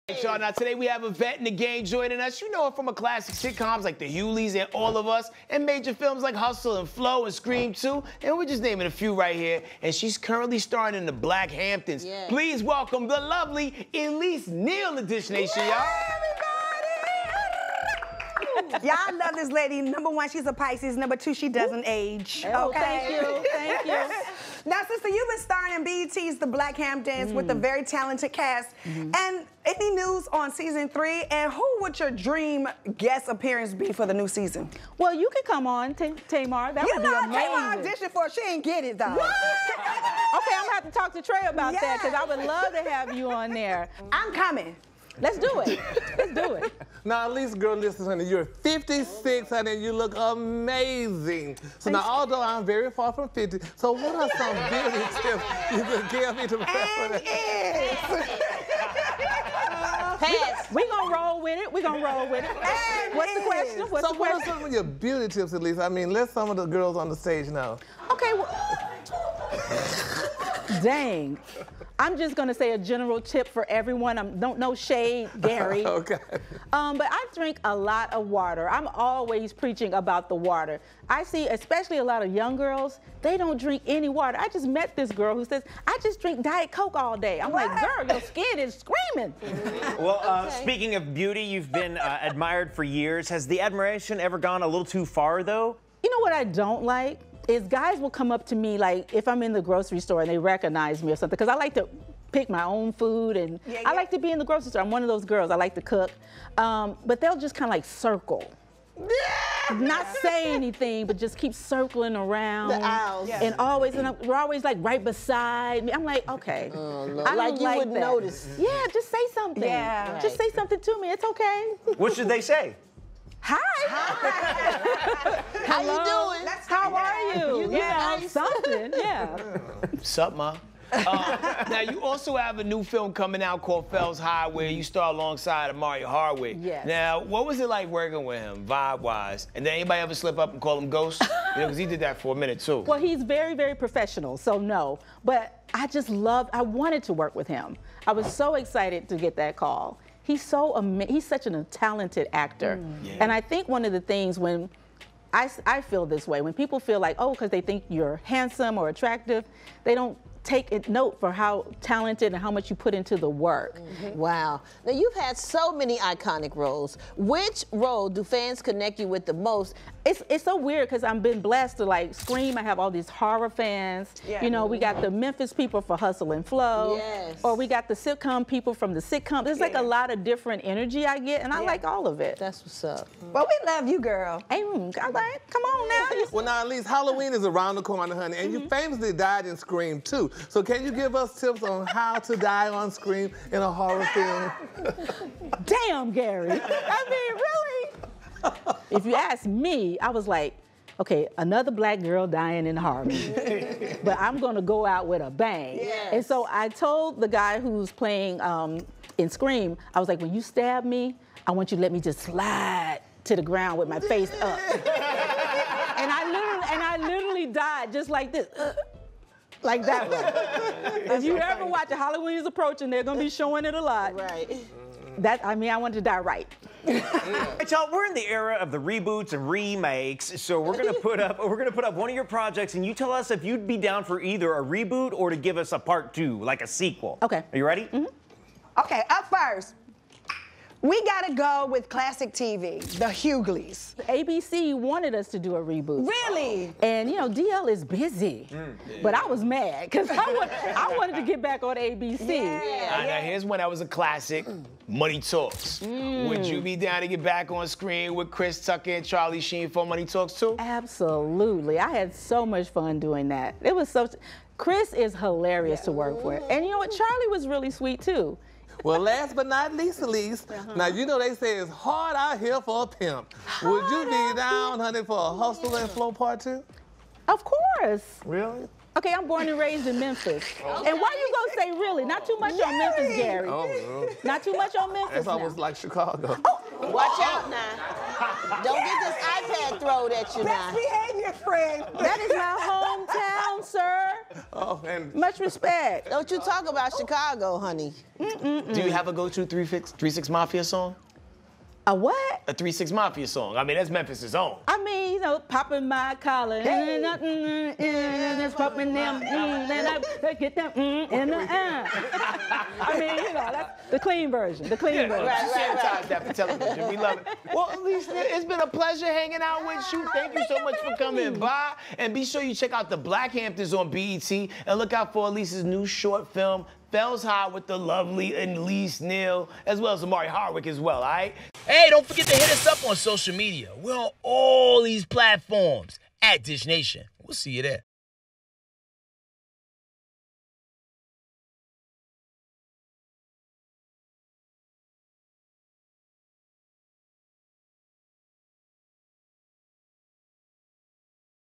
Dishin' It with Elise Neal: From Movie Sets to BEAUTY Tips!
Dishin’ it with Elise Neal! 🌟 She's giving us the tea on her career, upcoming roles, and her beauty secrets! Neal shared that her upcoming projects have “pushed her to new creative heights” and dishes on her upcoming roles. DishFam, do you have a favorite Elise Neal moment?